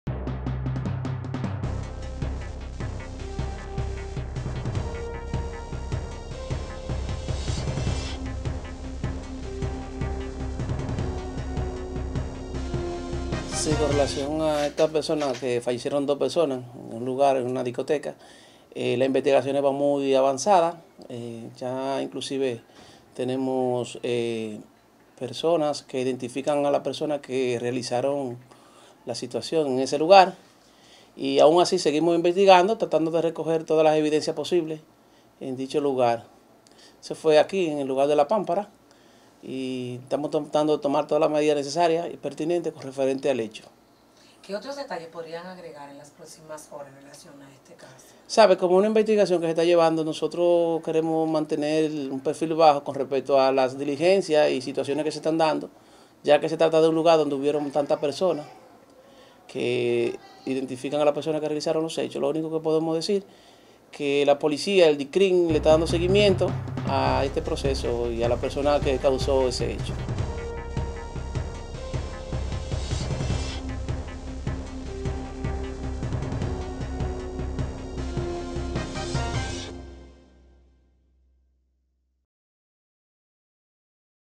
Ministerio Publico afirma avanzan investigación tras muerte de dos jóvenes en negocio de SFM
San Francisco De Macorís.- El Ministerio Público informó que avanzan las investigaciones sobre una balacera que se produjo en el lugar de expendio de bebidas conocido como "La Pampara" en el que dos personas murieron y otra resultó herida.

El magistrado Elinton Santiago Villar, dijo que se investigan las circunstancias en que fallecieron los nombrados Jonathan Martin Martínez Ciriaco y Dalfri Joel Recio, donde además resultó herida la joven Sudayny Gabriel Padilla, en el incidente ocurrido en el bar ubicado en el sector San Vicente.

Recordemos que la tragedia se produjo durante un tiroteo registrado la madrugada del pasado domingo 8 de mayo.